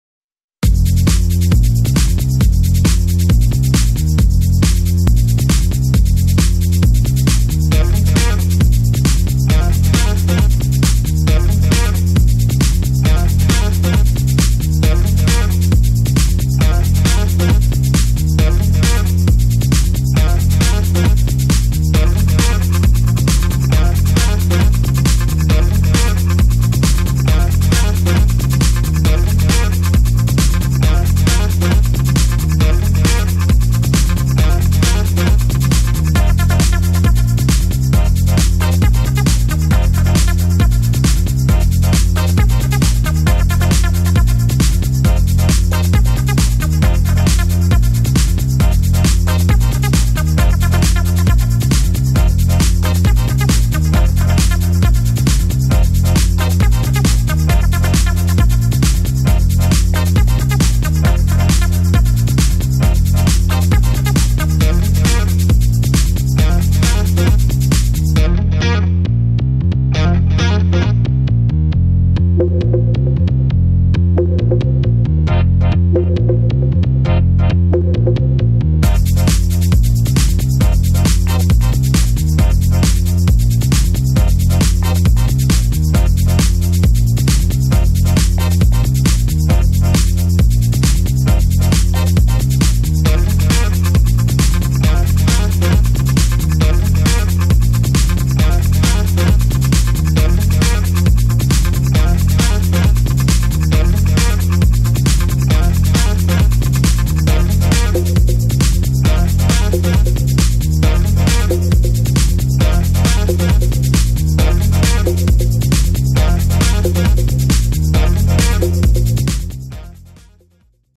Harry Houghton Rigging Demoreel 2020
This video shows some of the professional work I have worked on since 2015.  Below you can find the links to the cinematics shown in the order that they appear in my demoreel.